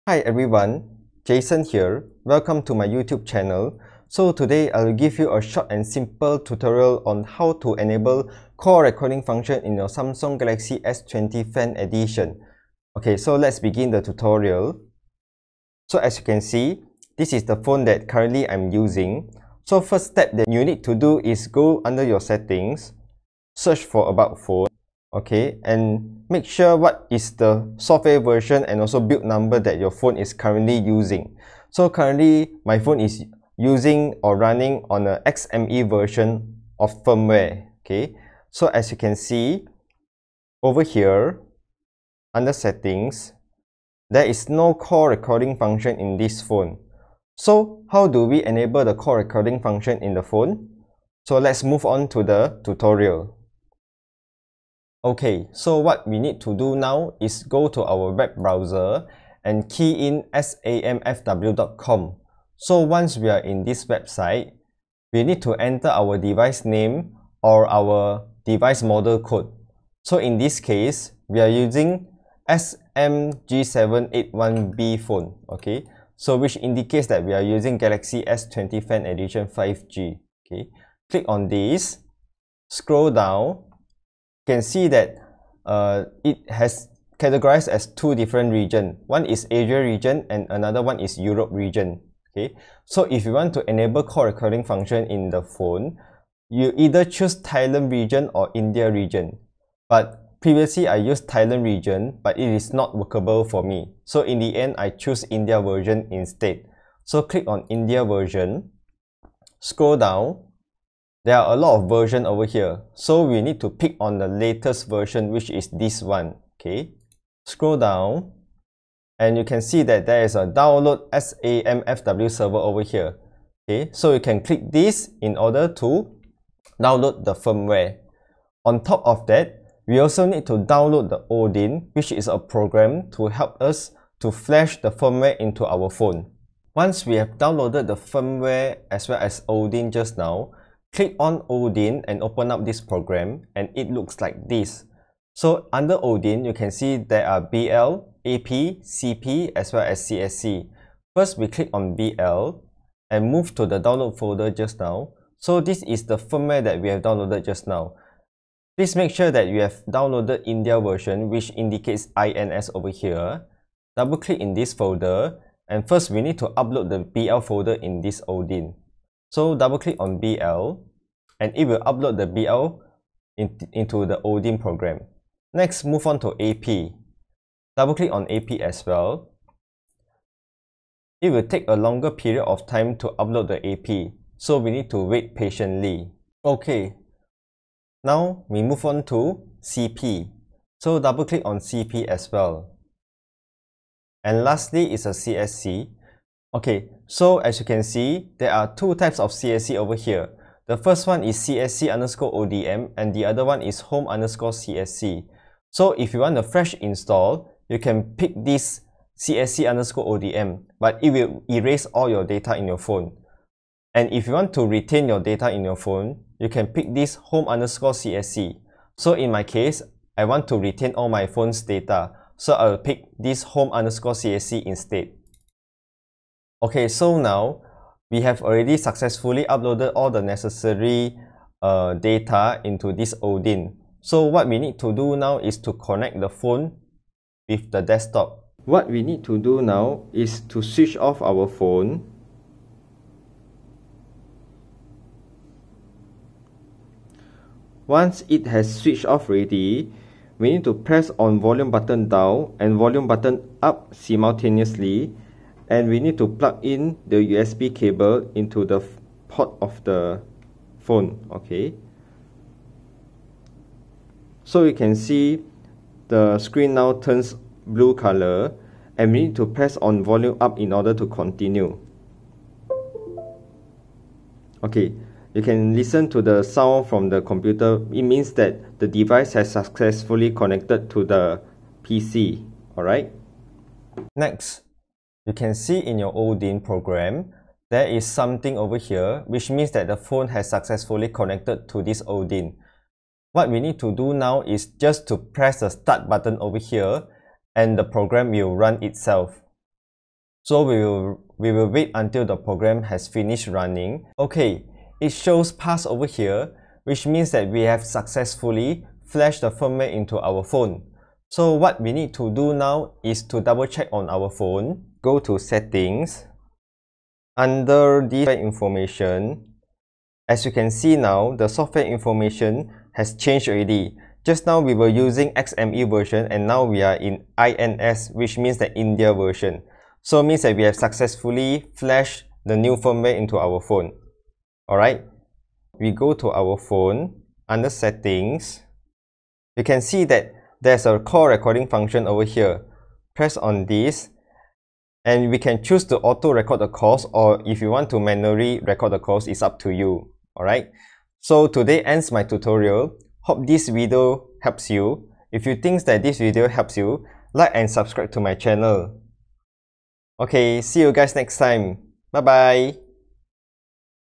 How to Enable Call Recording on Samsung Galaxy S20 FE 5G!!! NO ROOT REQUIRED!!! Working in 2025
You can visit these website to flash the firmware

Additional Reading Info